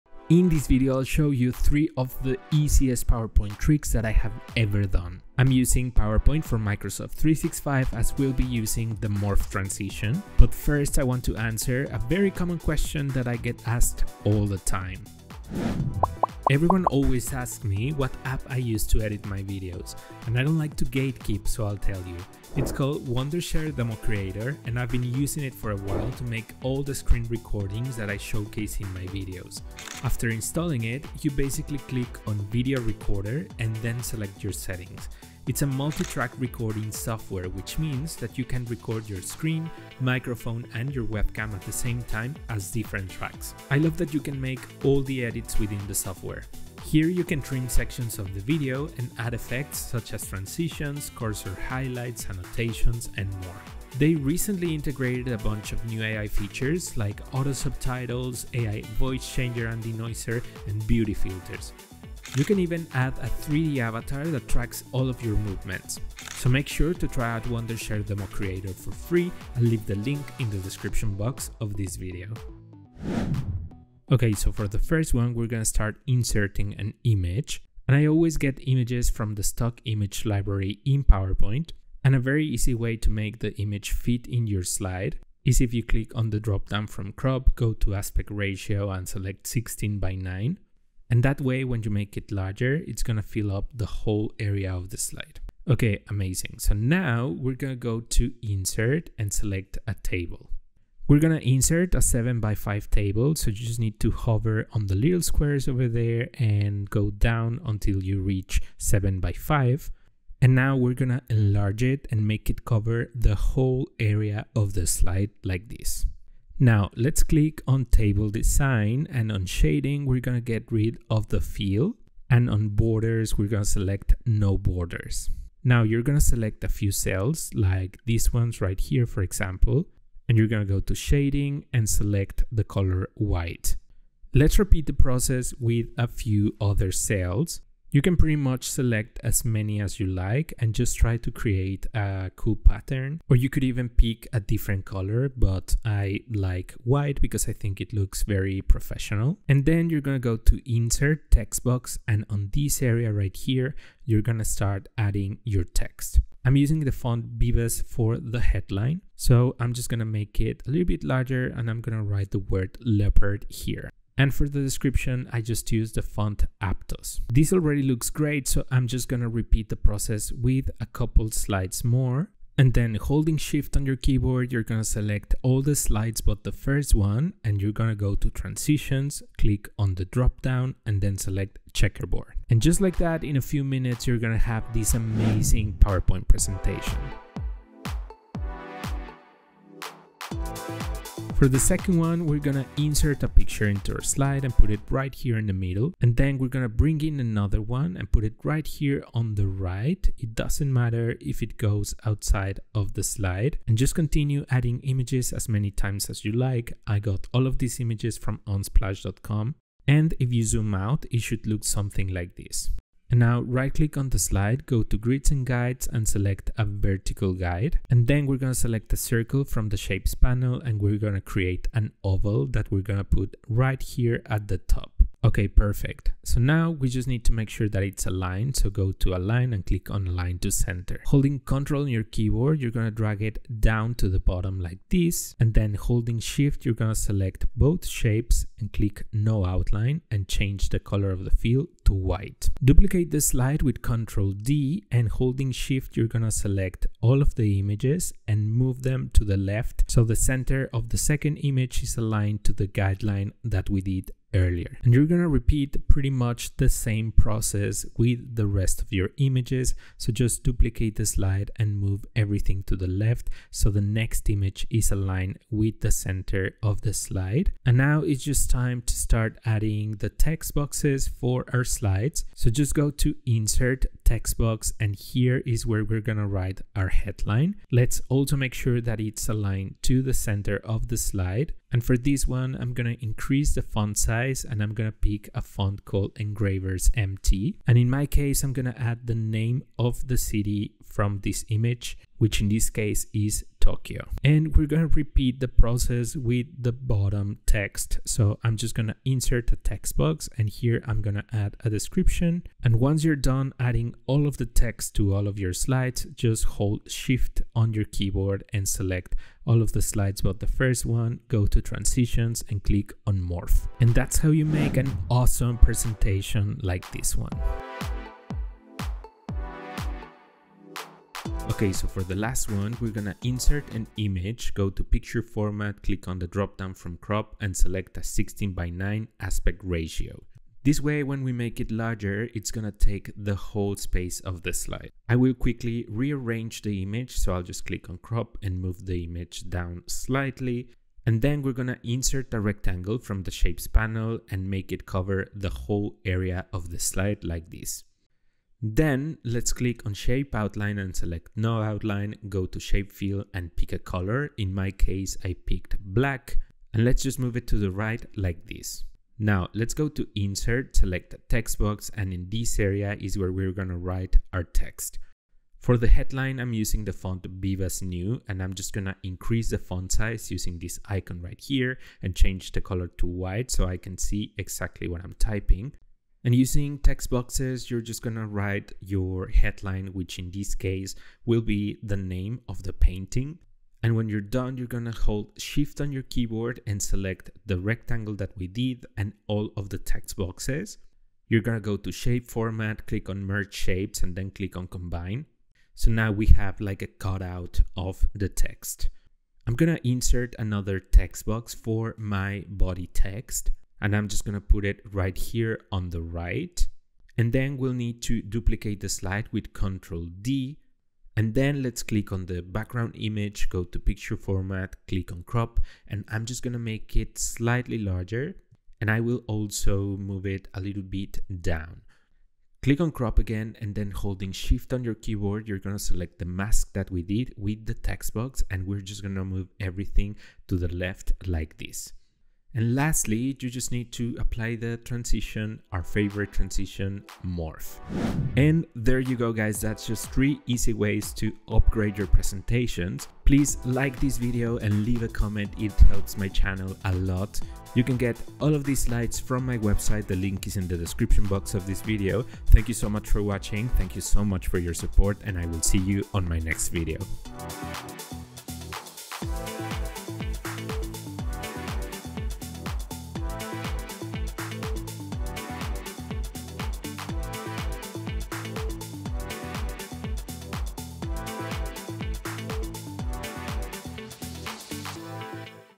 Three EASY PowerPoint Tricks to upgrade any presentation 🤯
Stop wasting hours redesigning slides — generate high-quality diagrams for your PPTs automatically with AI

Discover how to create three of the easiest yet most impactful PowerPoint animations and transitions in this step-by-step tutorial. Whether you're a beginner or a seasoned pro, you'll learn how to quickly enhance your presentations with stunning effects. Perfect for anyone looking to make their slides more engaging and visually appealing. Don't miss out on these simple tricks to take your PowerPoint skills to the next level!

Chapters:

0:00 - Intro
00:16- Democreator
01:13- Mosaic Trick
03:11- Carousel Trick
05:21- Mask Trick
07:19- Result